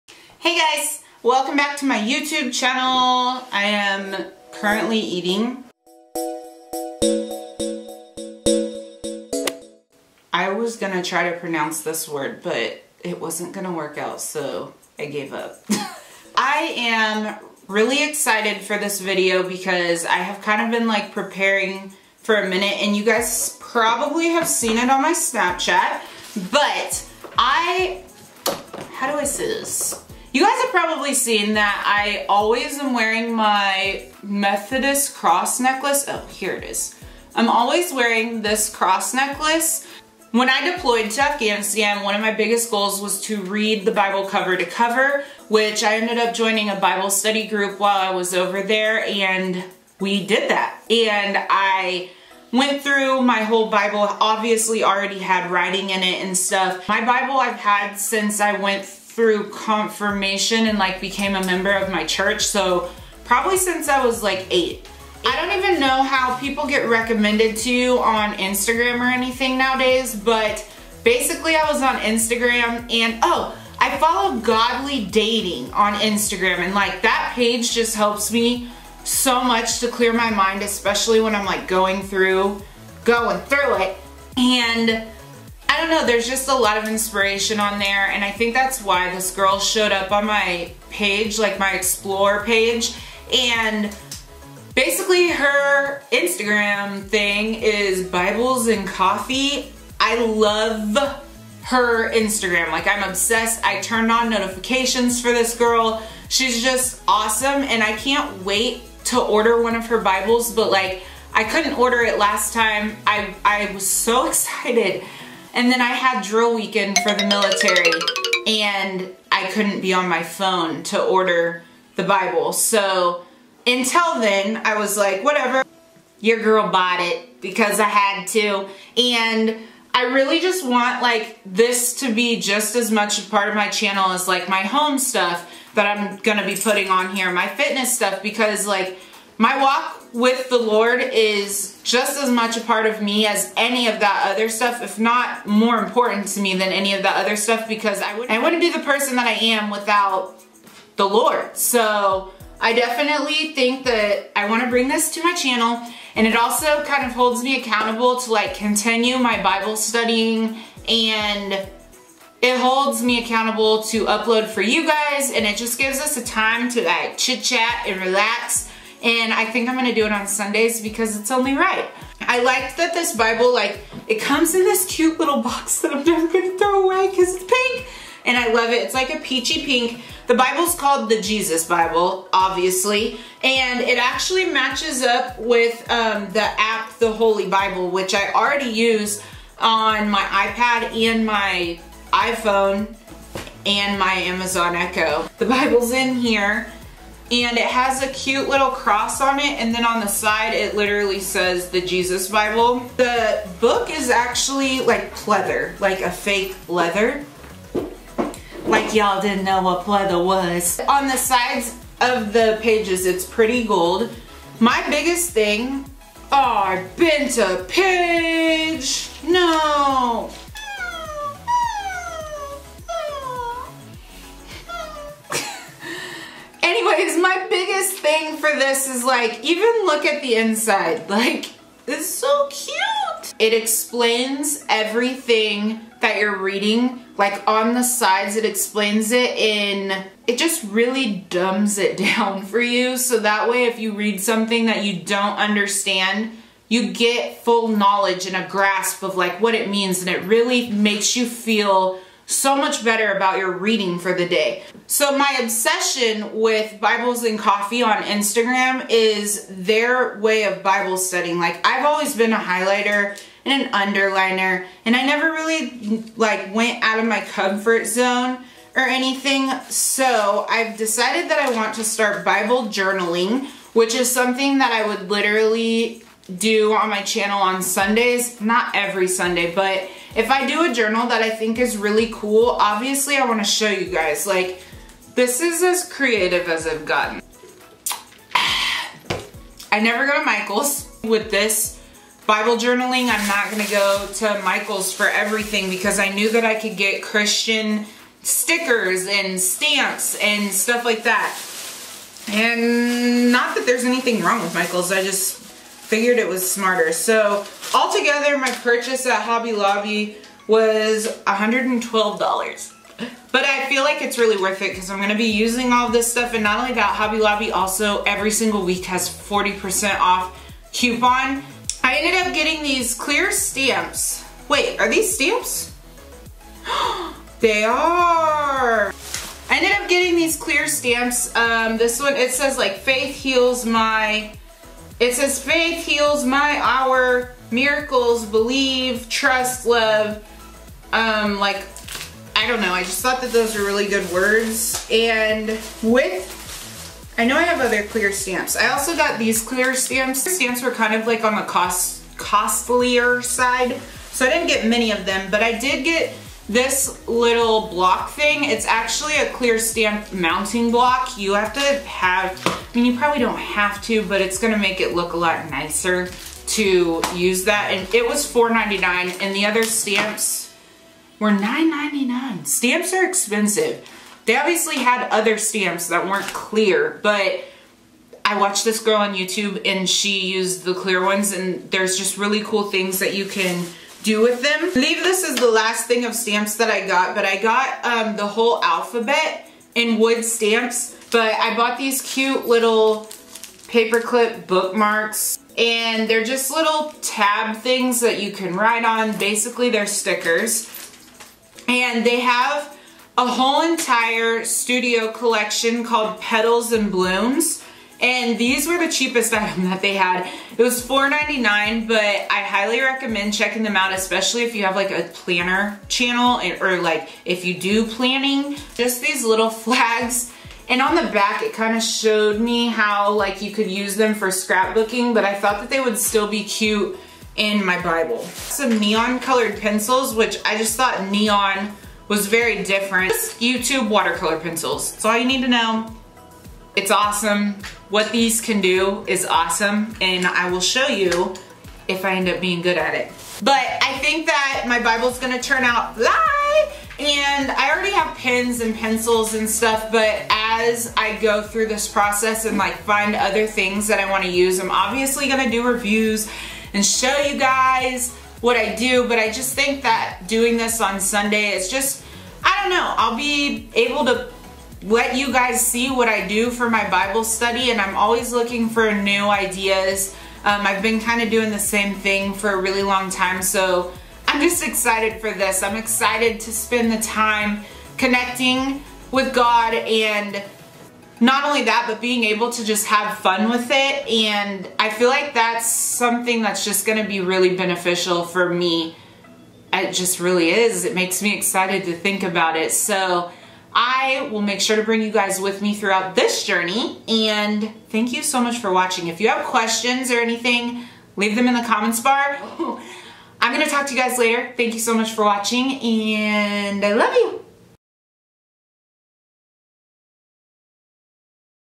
|JOURNALING to study the BIBLE?|
VIDEO FAQ BELOW.  UPGRADE TO HD FOR BETTER QUALITY.

Today I am bringing you a haul of items that I purchased at Hobby Lobby as a beginners kit to Bible journaling.  I really fell in love with this idea of journaling for doing my bible study.  I will do this weekly to really grow in my faith with the Lord, and I totally want you to join me!  Below are the Instagram pages that inspired me.